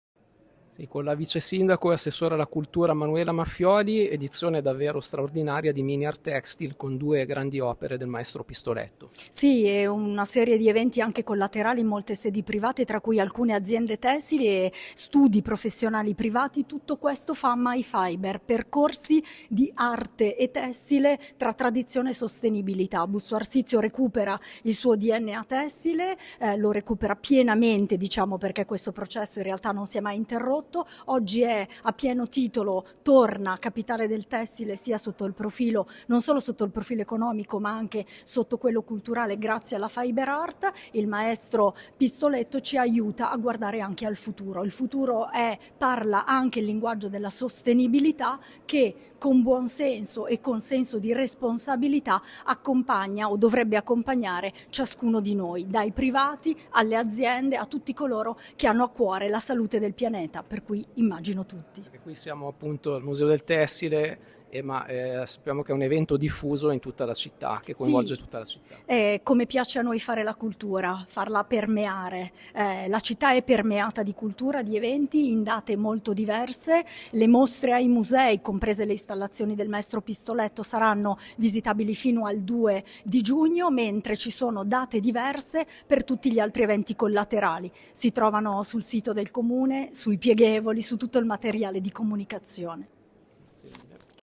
Busto Arsizio – Arte e tessile, un mese di eventi
L’assessore Maffioli: “Busto recupera il suo Dna tessile”